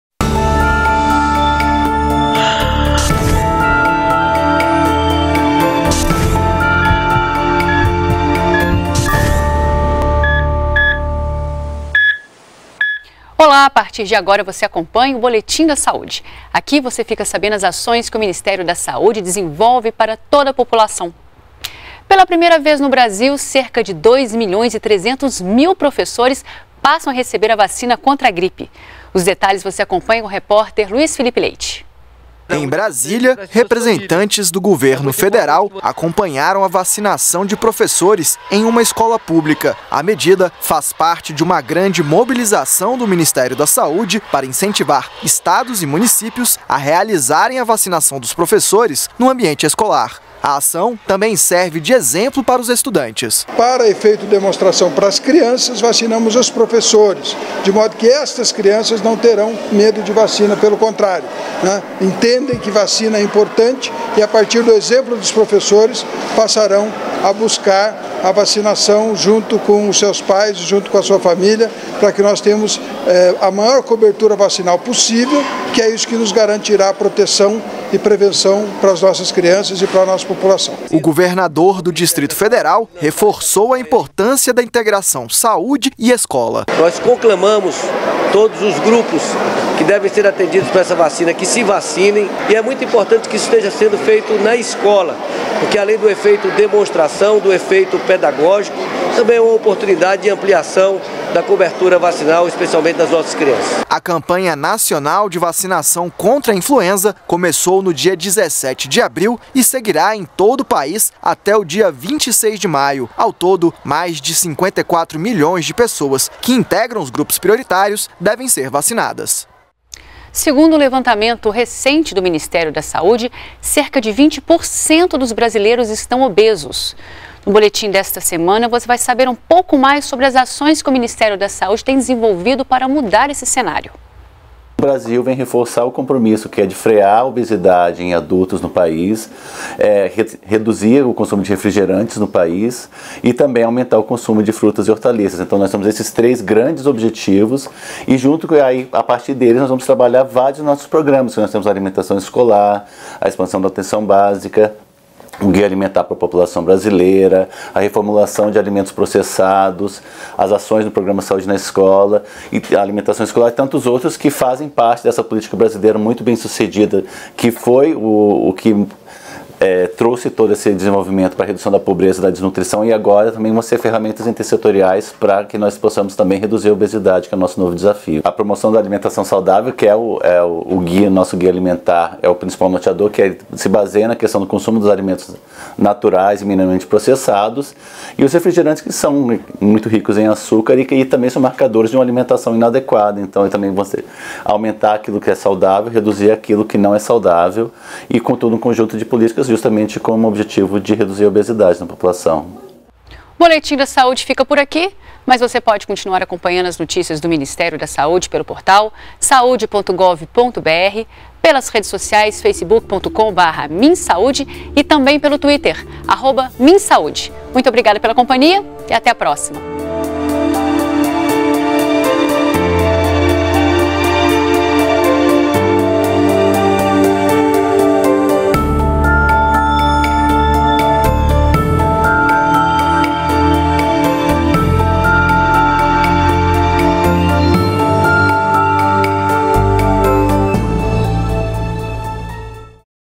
Boletim da Saúde - 5 de maio
BOLETIM DA SAÚDE - 05.05.17: Ministério da Saúde incluiu mais de dois milhões de professores na campanha de vacinação contra a gripe. Pesquisa mostra que 20% dos brasileiros estão obesos.